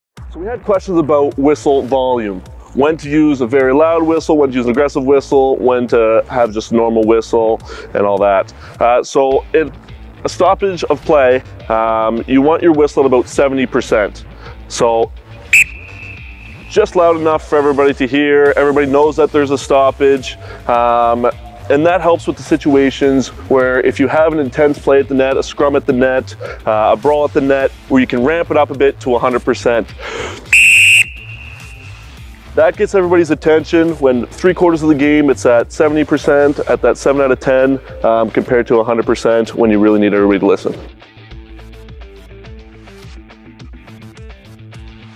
Blowing your Whistle | Tips for Hockey Referees
Blowing the whistle is pretty straightforward right? Believe it or not there is an art to it. In this video Junior A Referee Alex Estabrooks shows us that depending on the situation in the game, you may approach blowing the whistle differently.

Want to learn more? for more tips and tricks to improve your officiating.

You can even check out our Academy with instruction from NHL, AHL, and other top officials!